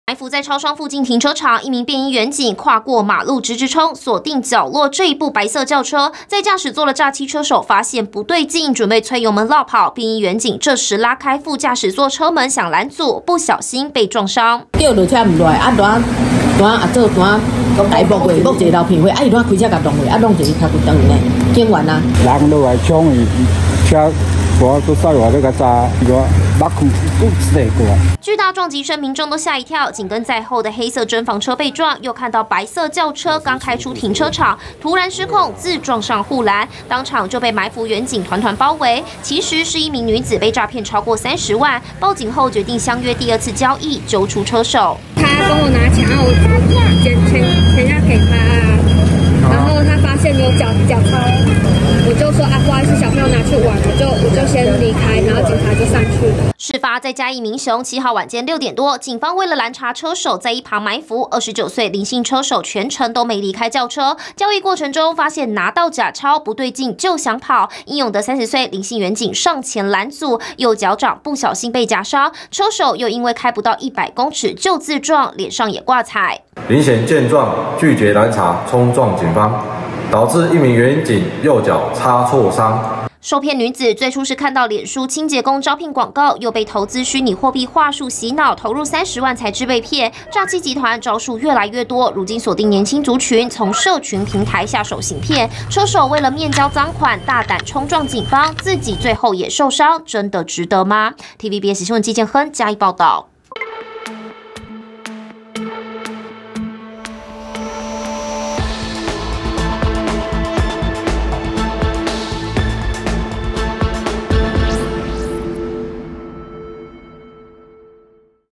詐團車手被圍捕開車撞警!害警腳受傷 他自撞也掛彩｜TVBS新聞
實在太囂張！嘉義民雄發生撞警事件，一名女子遭詐騙30萬，相約車手第二次面交，警方在圍捕過程中，被車手開車衝撞，導致員警右腳受傷流血，車手也因為失控，自撞護欄，臉部受傷。